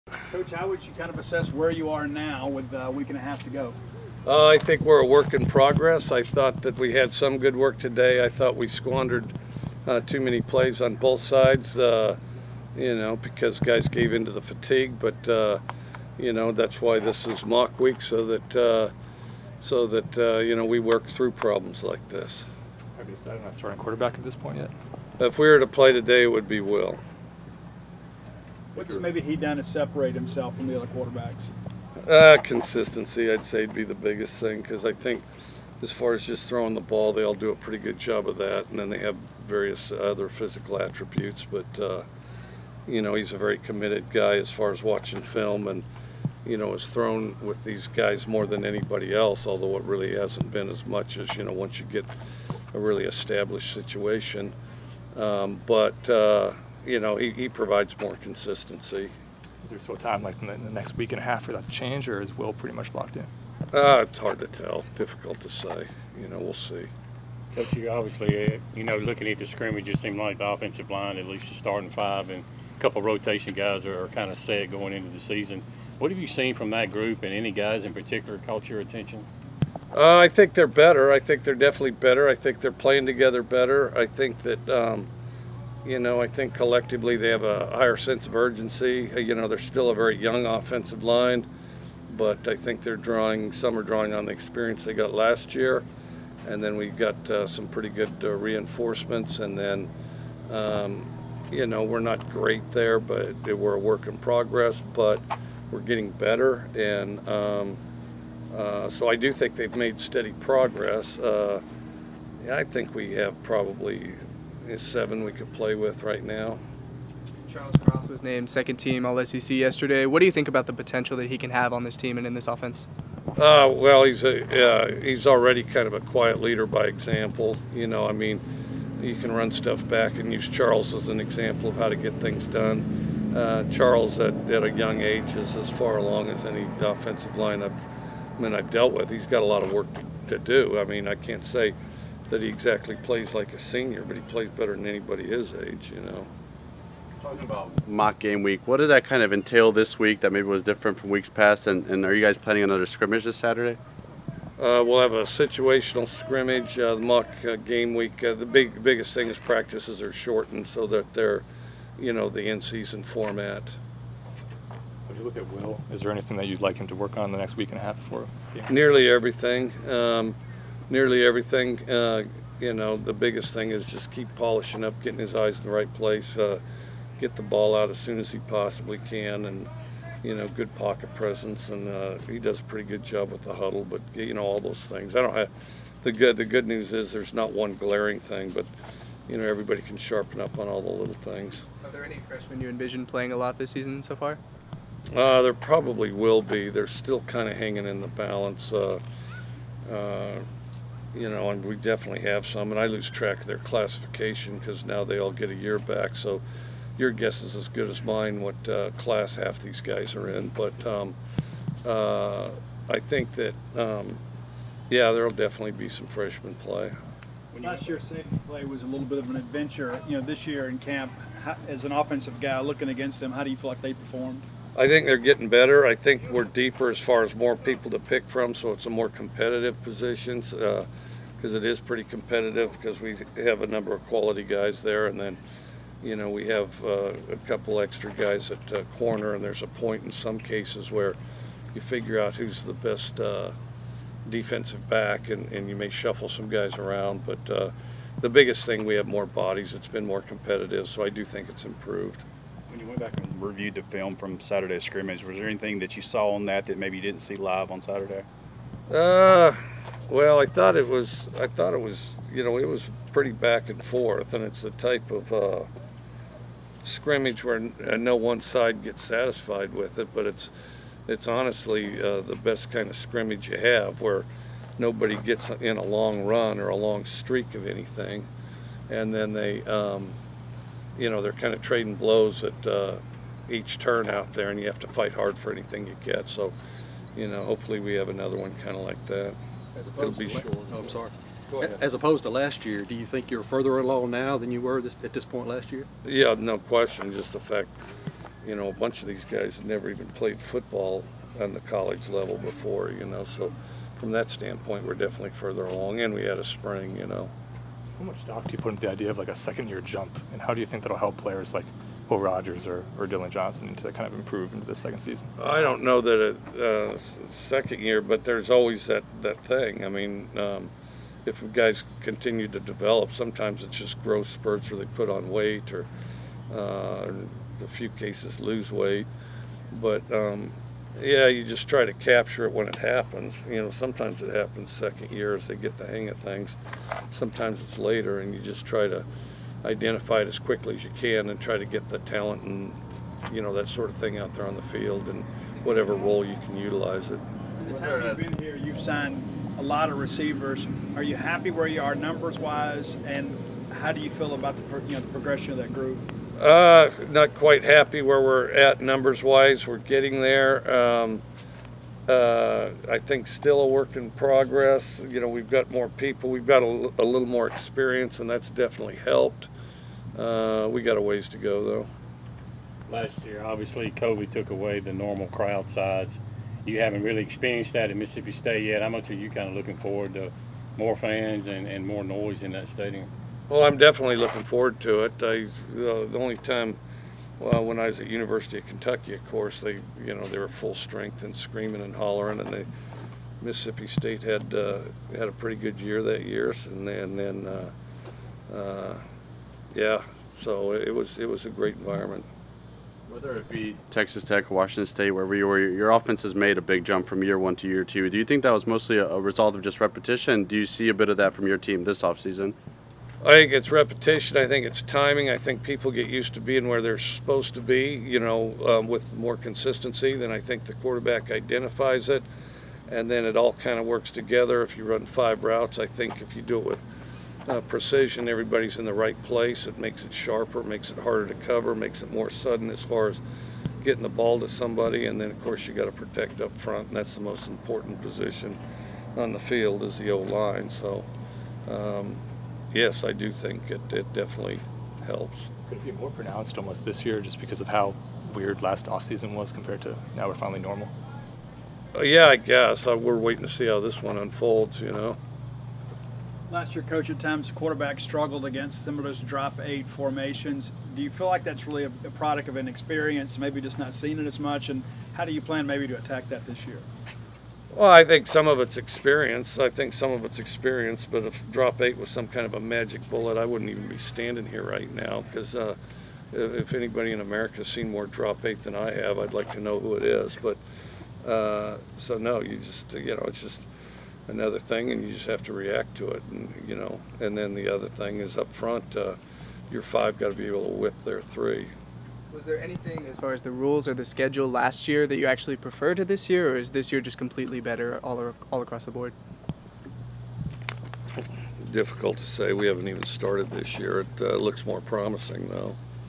Mike Leach Mississippi State Bulldogs recaps end of training camp; previews 2021 opener August 25
Video from Mississippi State Athletics